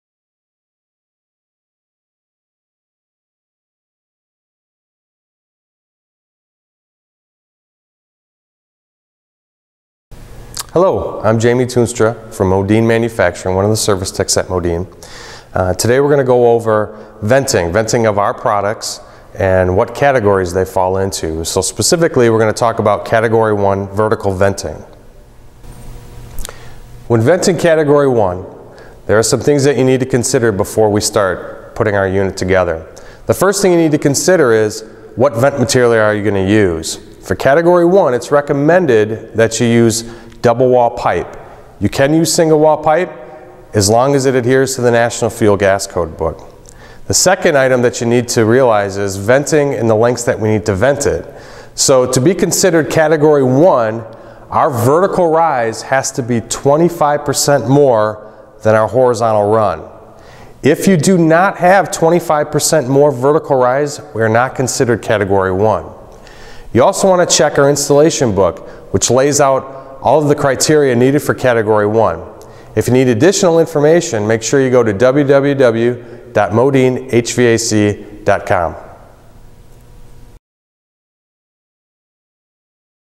How to Vent a Modine Unit Heater Category I Vertically
Watch as one of Modine's service tech's discusses how to vent a Modine unit heater category I vertically.